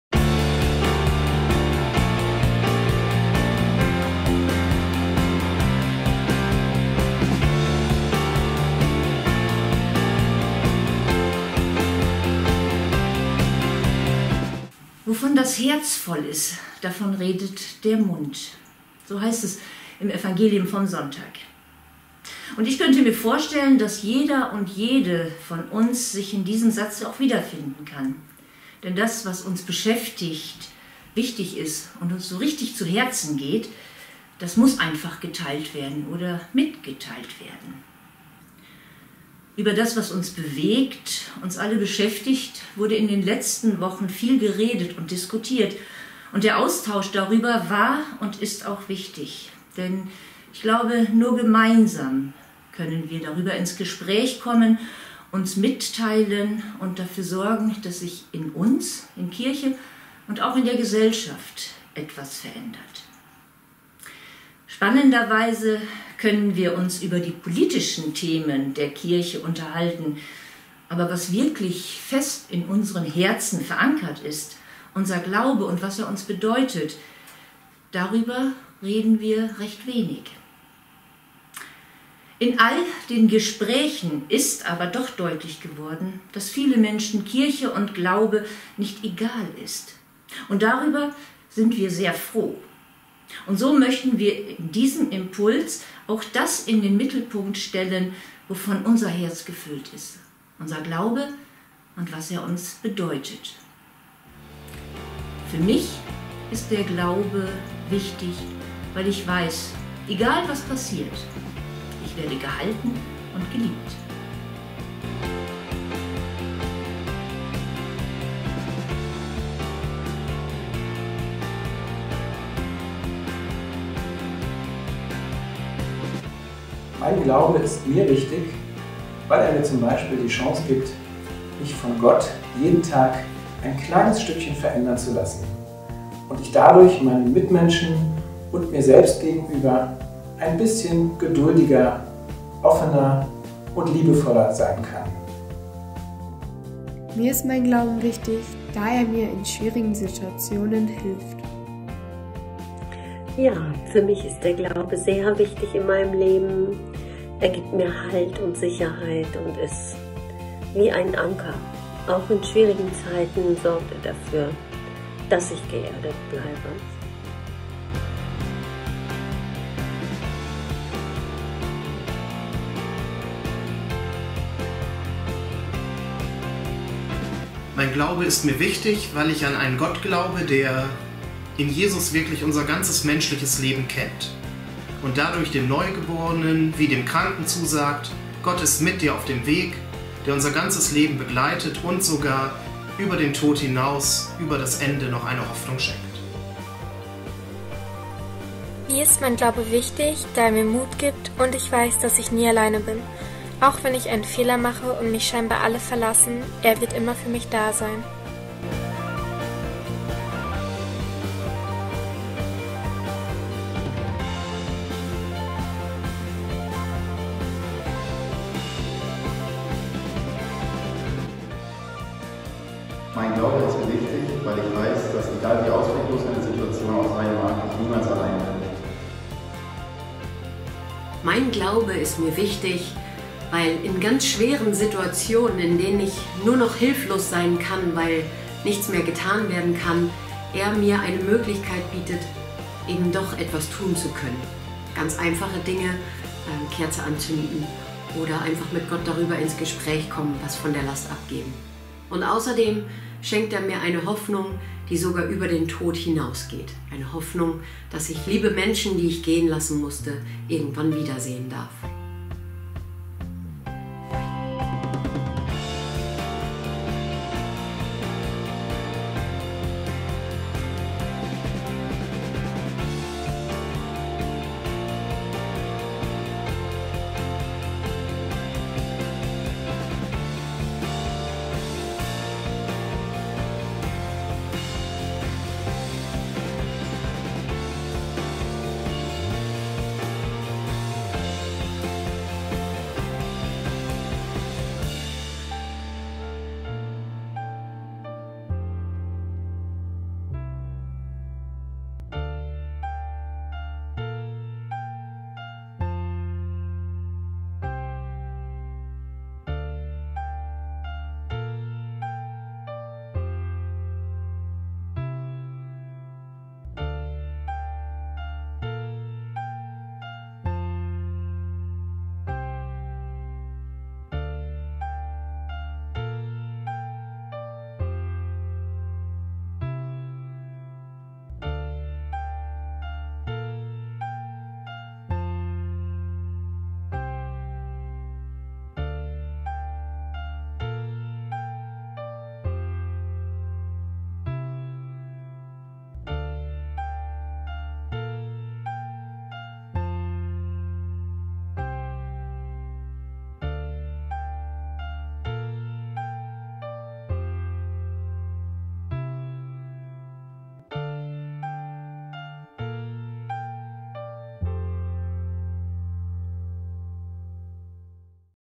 Impuls zum Wochenende 25.-27.02.2022: Mein Glaube ist mir wichtig
Die Kirche ist derzeit viel im Gespräch. Viele tauschen sich über wichtige kirchenpolitische Themen aus. Aber über den Urgrund all dessen, über unseren Glauben, reden wir eher nicht so viel. In diesem Impuls hören wir, was unser Herz bewegt: Wir hören von unserem Glauben, und was er uns bedeutet...